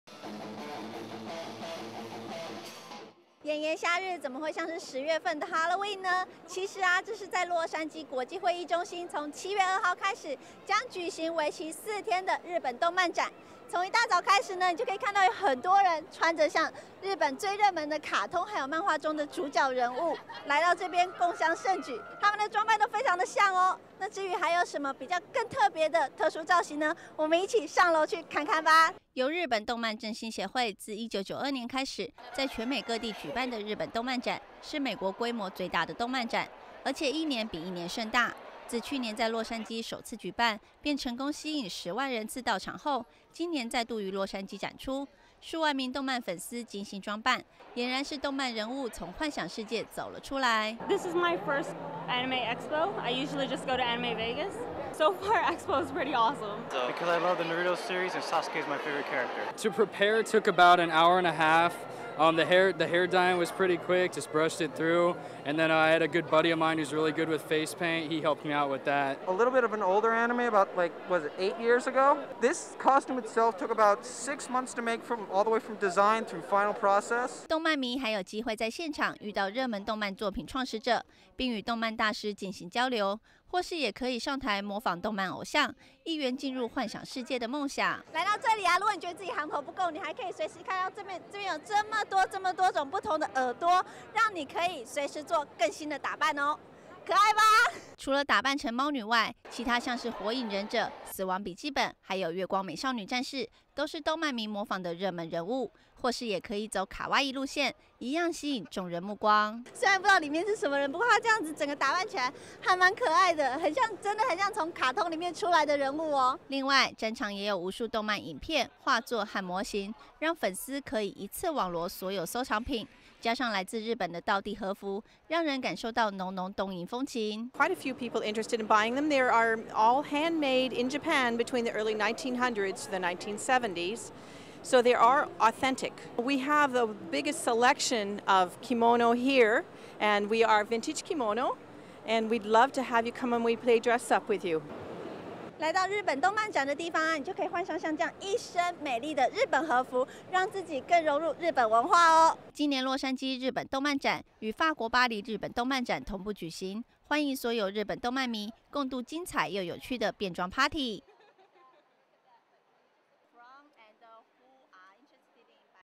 2009年洛杉磯日本動漫展
匯聚無數日本熱門動漫作品的「日本動漫展」，每年舉辦期間總是吸引來自全美及日本的眾多動漫粉絲齊聚一堂，享受一場動漫盛宴，來到展場不但有機會遇見日本動漫大師，還有機會參加動漫偶像模仿大賽，活動內容相當豐富有趣。